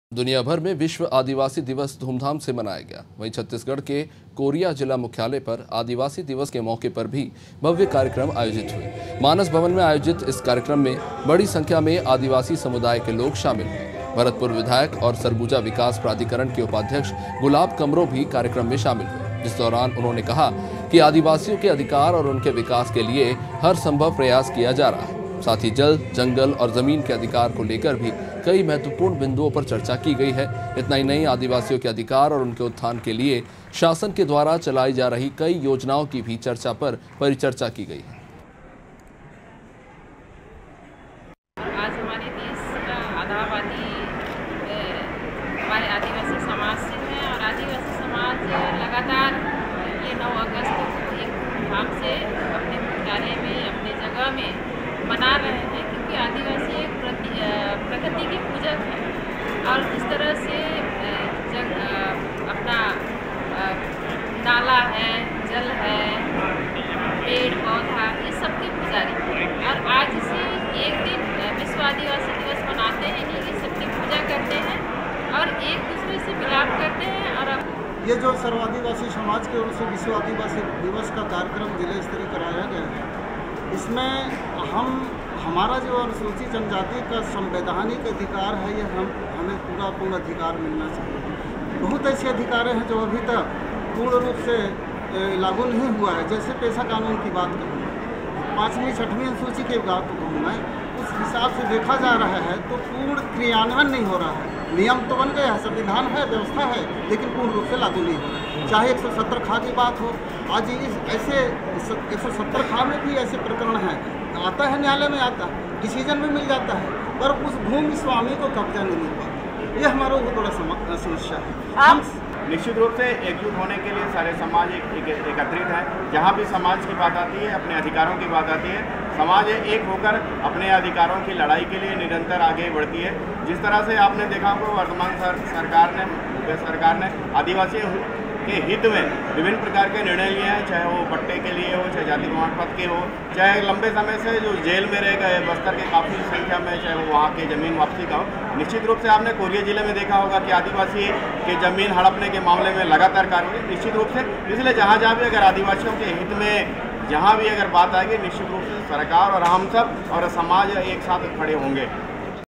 कोरिया: विश्व आदिवासी दिवस मनाया| Top News today Hindi | Breaking News Hindi |mp news live
दुनिया भर में विश्व आदिवासी दिवस धूमधाम से मनाया गया...वहीं छत्तीसगढ़ के कोरिया जिला मुख्यालय पर आदिवासी दिवस के मौके पर भी भव्य कार्यक्रम आयोजित किया... मानस भवन में आयोजित इश कार्यक्रम में बढ़ी संख्या में आदिवासी समुदाय के लोग शामिल हुए...भरतपुर विधायक और सरगुजा विकास प्राधिकरण के उपाध्यक्ष गुलाब कमरों भी कार्यक्रम में शामिल हुए...इस दौरान उन्होंने कहा कि आदिवासियों के अधिकार और उनके विकास के लिए हर संभव प्रयास किया जा रहा है...साथ ही जल, जंगल और जमीन के अधिकार को लेकर भी कई महत्वपूर्ण बिंदुओं पर चर्चा की गई...इतना ही नहीं आदिवासियों के अधिकार और उनके उत्थान के लिए शासन के द्वारा चलाई जा रही कई योजनाओं की भी चर्चा पर परिचर्चा की गई...